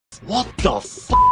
wut the F--k?
my reaction to some of the content on ponibooru

my little pony is copyright to hasbro